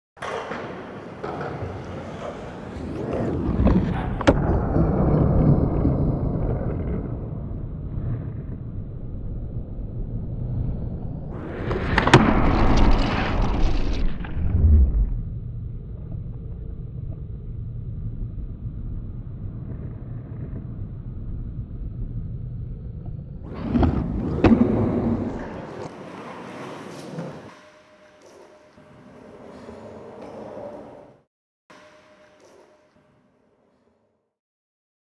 PJ LADD "PRO.SPEC" PLAN B WHEEL COMMERCIAL
PLAN B INTRODUCES PRO.SPEC WHEELS
ZERO FLATSPOTS. 100% GUARANTEED

PJ Ladd takes his new wheel out for a spin, with a fakie 5-0 to fakie 360 flip.

Filmed on the Phantom Flex camera, and edited by Kirk Dianda.